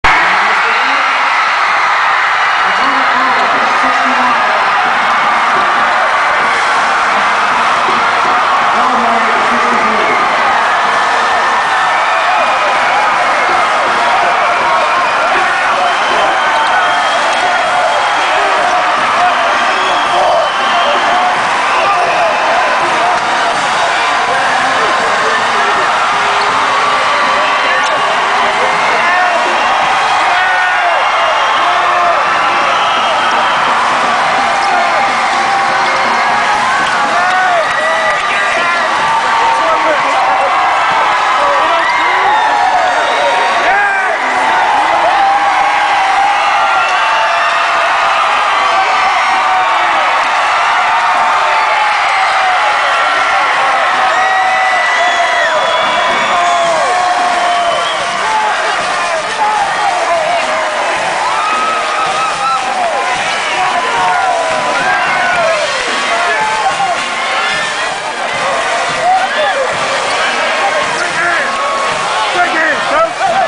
Potomac Falls High School basketball Loudoun County
The Potomac Falls Panthers celebrate their first state basketball championship following a 69-64 victory over Broadway in double overtime March 13 at Virginia Commonwealth University's Siegel Center.